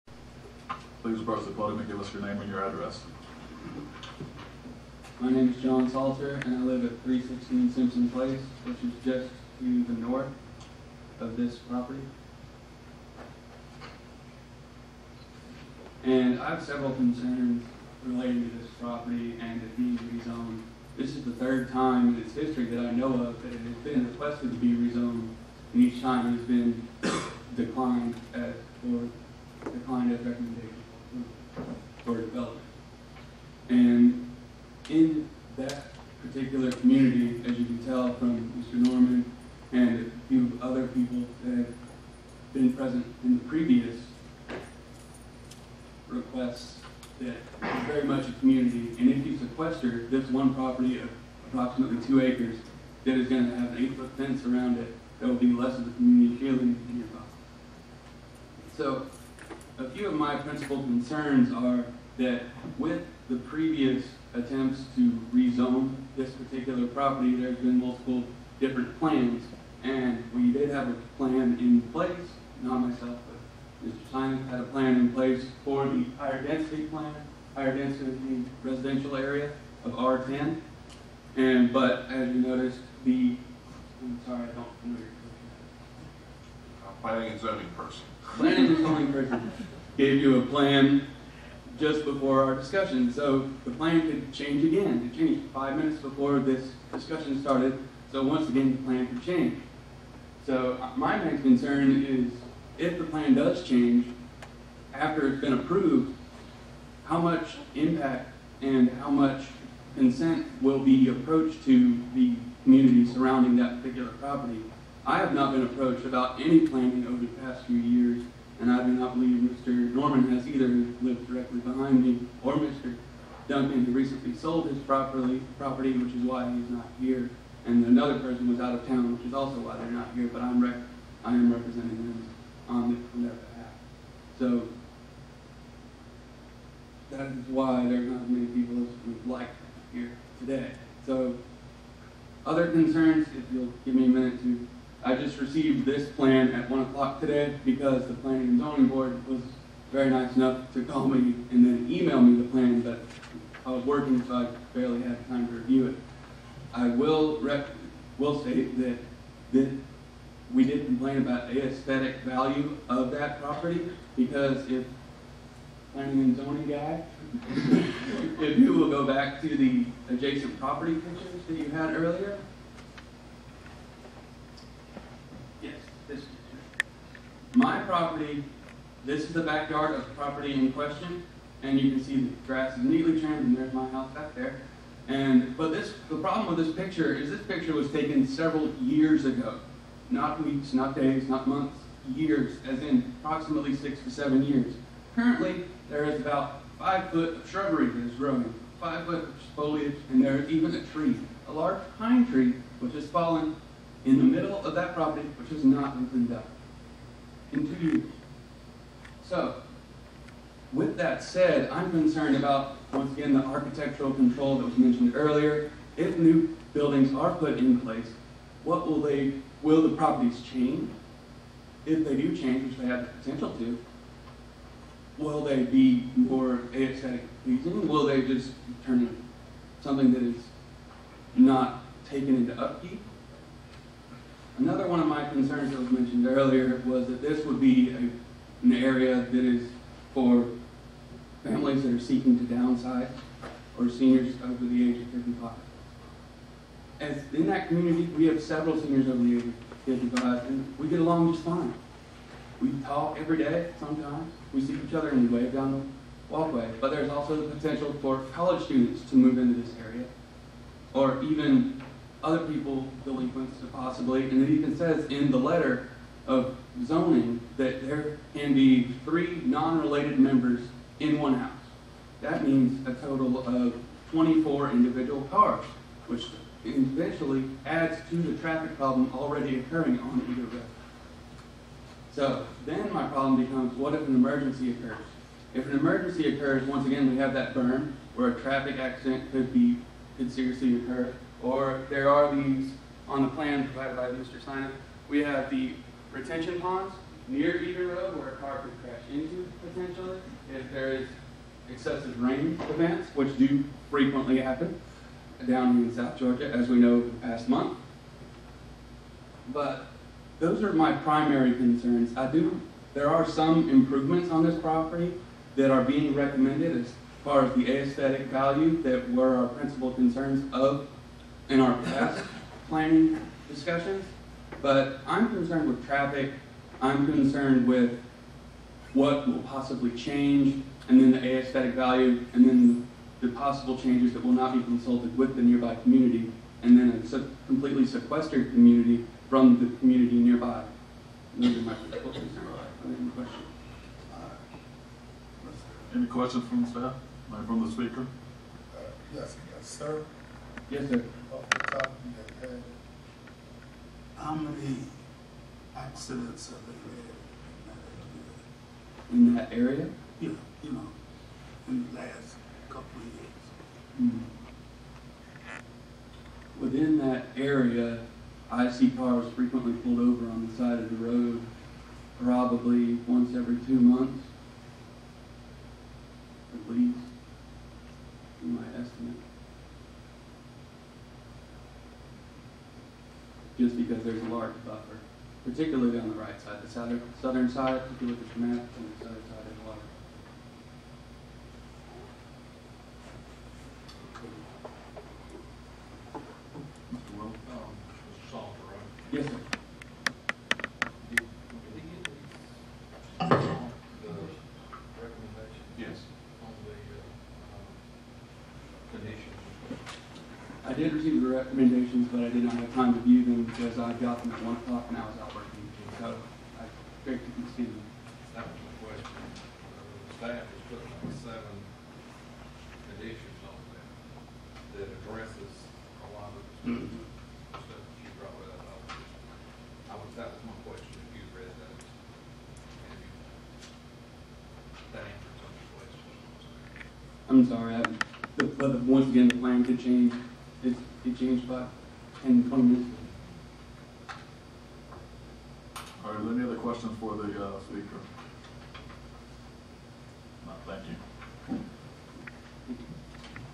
6. VA-2013-10 Tombrooks, LLC (8) Public Hearing - GLPC 2013-07-29
Greater Lowndes Planning Commission (GLPC)
6.      VA-2013-10      Tombrooks, LLC
Property Location: 316 Eager Road, Valdosta, Georgia
Request to rezone 1.8 acres from Single-Family Residential (R-15) to Single-Family Residential (R-10)
Video by Gretchen Quarterman for Lowndes Area Knowledge Exchange (LAKE).